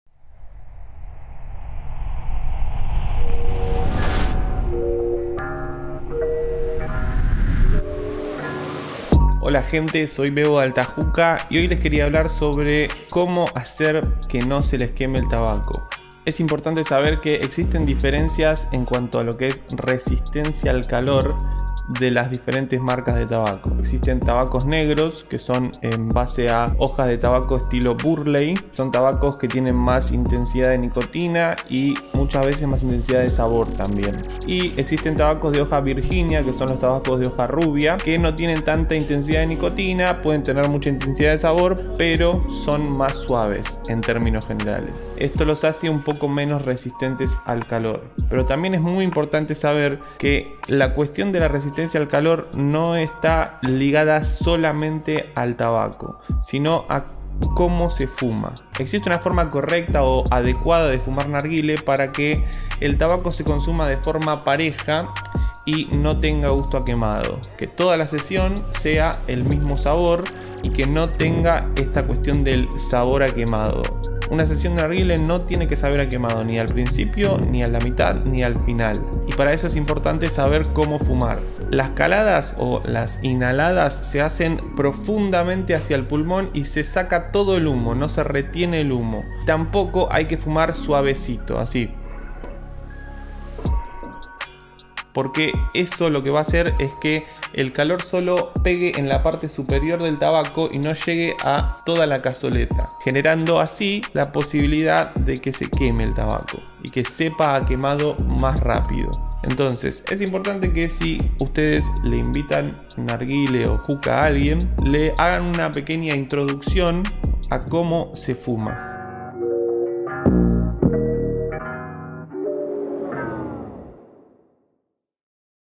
AH TIPS - COMO FUMAR HOOKAH 🇦🇷
Hookah TIPS #1 - COMO FUMAR HOOKAH 🇦🇷
Si bien es real que ciertos tabacos pueden tener distintas resistencias al calor, es un factor muy importante y ciertas veces ignorado, el hecho de "cómo" se fuma la hookah.
Las caladas deben ser realizadas en profundidad, exalando por completo el humo sea por nariz o boca.
De esta manera el calor atravesará el rosh por completo evitando el quemado superficial del tabaco.
📍 Local a la calle
🚐 Hacemos envíos a todo el país!! 🇦🇷
✉️ Consultas x DM